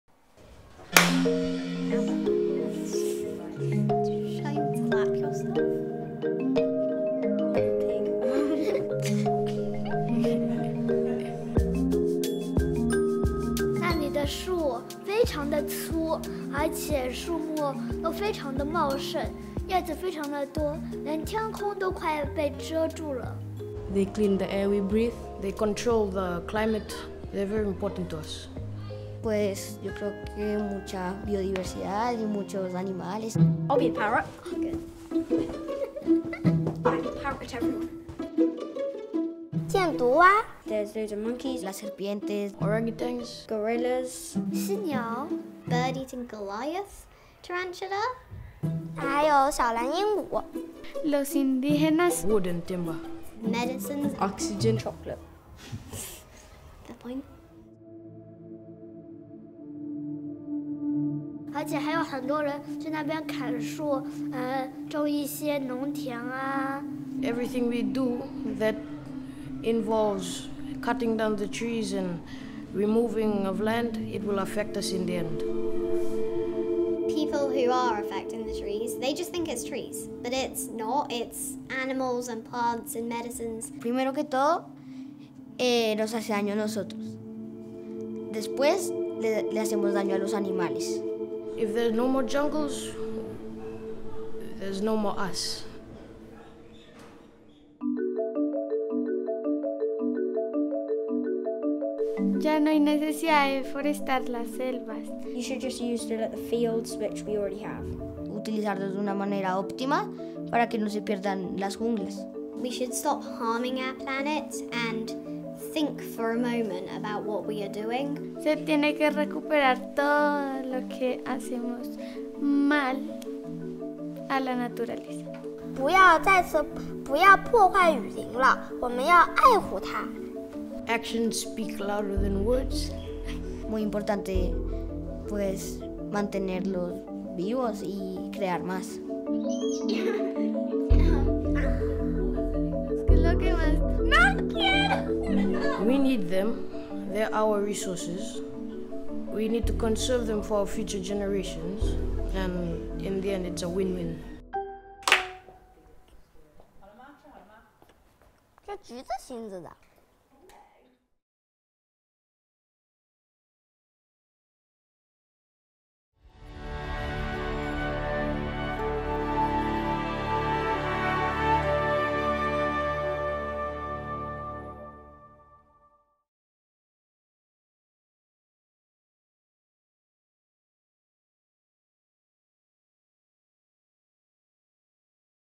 Schoolchildren explain why we need jungles
School children from around the world tell us why jungles are awesome and how we can keep them standing forever.